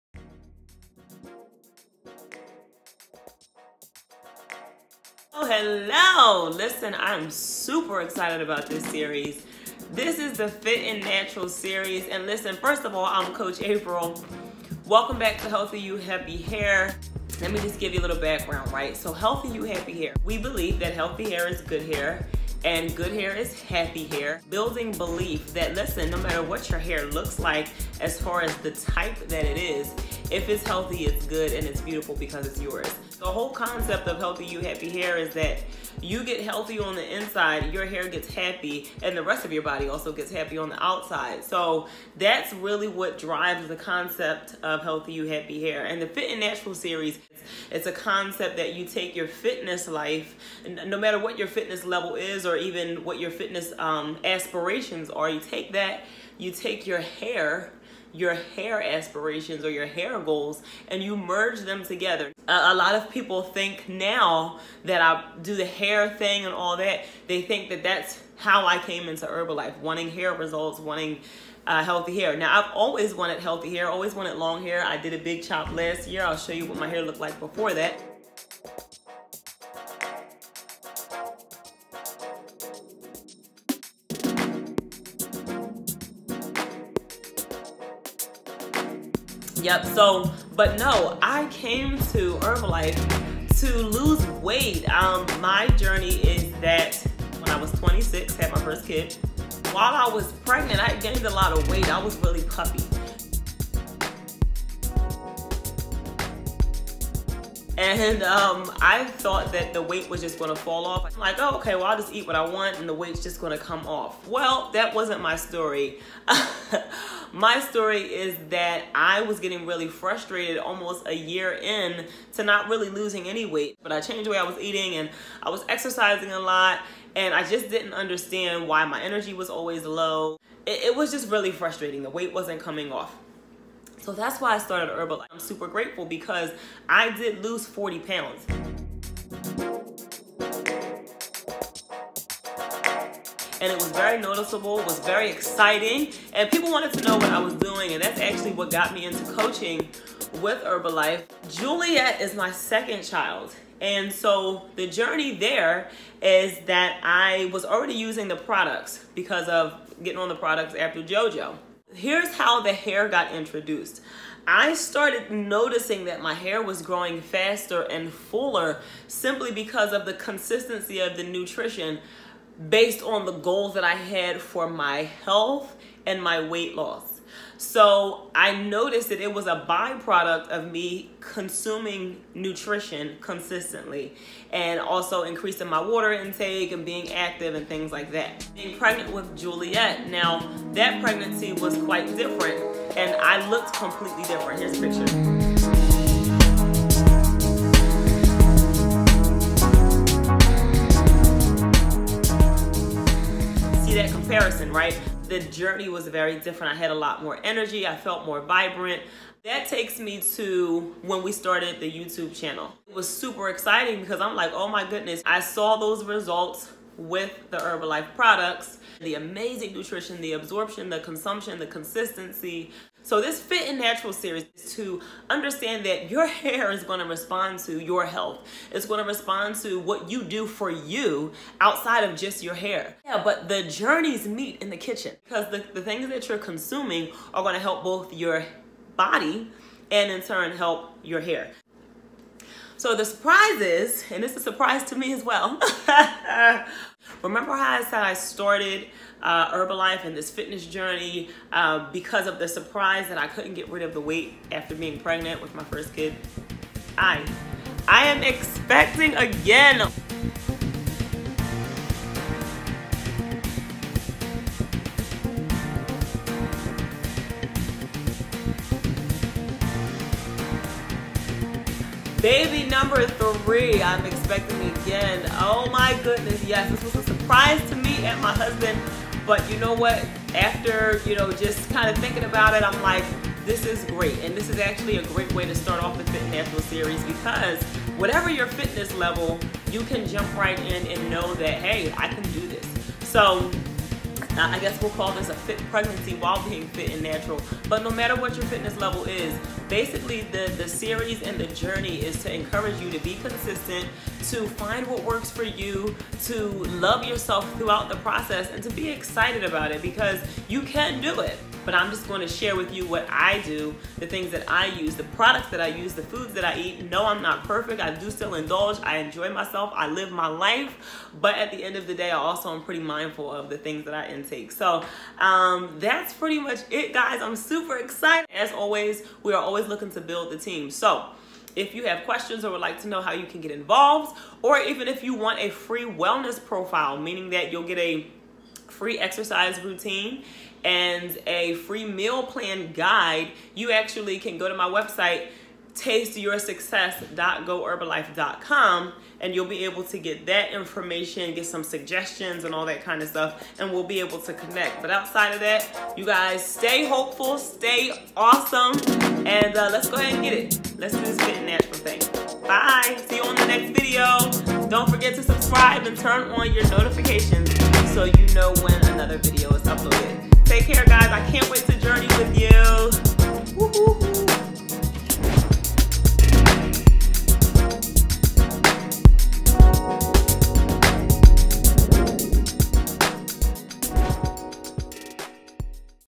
Fit & Natural Series: Let's Get It Started!
Thank you as always for coming through! This is the introduction to the Fit & Natural Series which merges the idea of increasing your fitness health and hair health all at the same time by putting emphasis on what will impact both journeys the most... Your Nutritional intake! It's a lot simpler than we make it sometimes... But our hope is that you will be inspired to KISS (Keep it Simple Sista) and enjoy the journey!! You'll also be able to see my timeline with the fitness, weight loss and why I started Herbalife! Yes, we'll workout and talk about hair ideas, but overall we will have fun getting better together!!! Let's get it!! Oh yeah and there's a "little" surprise at the end of the 🎉🎉